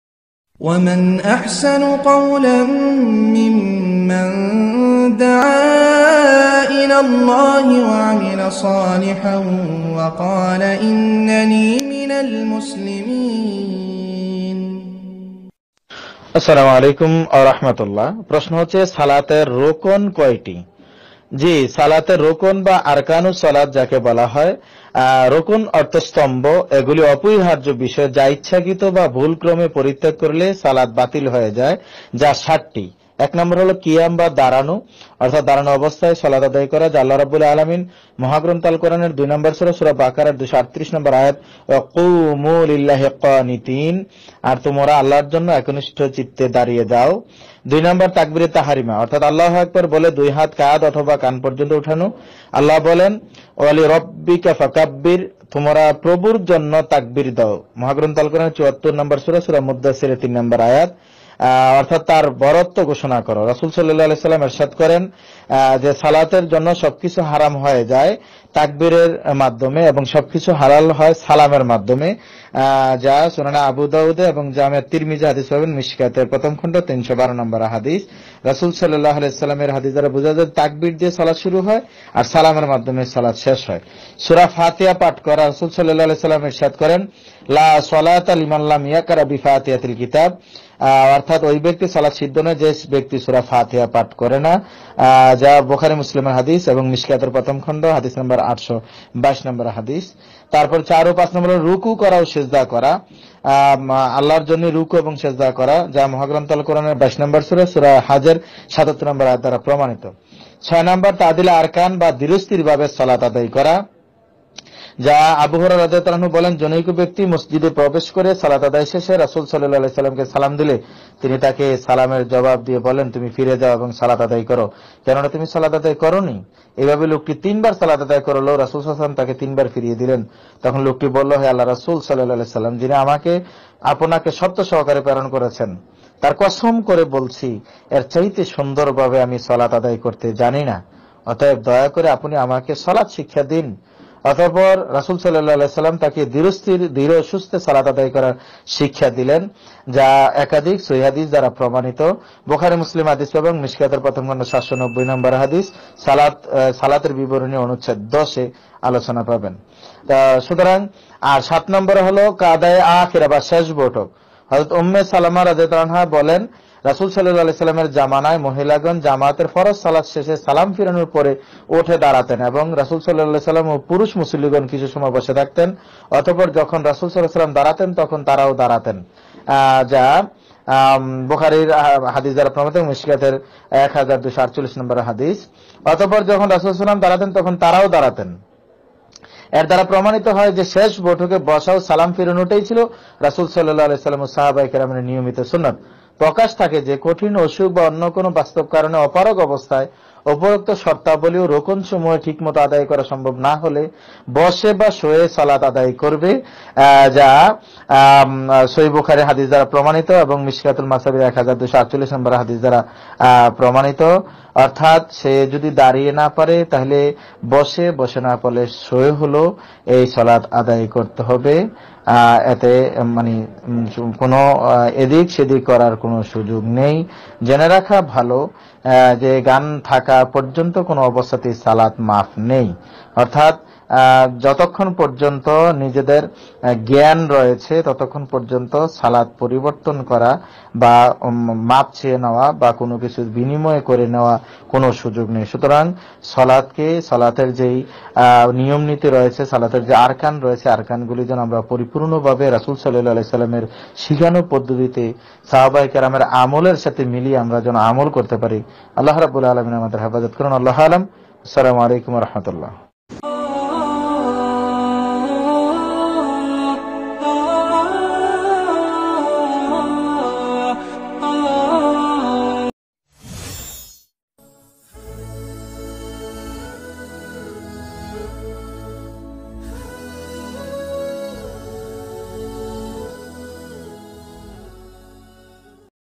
স্বলাতের রুকন কয়টি ও কি কি ? শাইখ মোহাম্মাদ শফিকুল ইসলাম।
স্বলাতের রুকন কয়টি ও কি কি ?

🎤 শাইখ মোহাম্মাদ শফিকুল ইসলাম।

এম,এম (এম,এ) হাদীস,ঢাকা।
বি,এ অর্নাস, এম,এ (ইসলামিক স্টাডিজ)
খ্বতীব, দোলেশ্বর আল-নূর জামে মসজিদ,ঢাকা।
আলোচক:- বাংলাদের টেলিভিশন ও বৈশাখী টিভি, ঢাকা।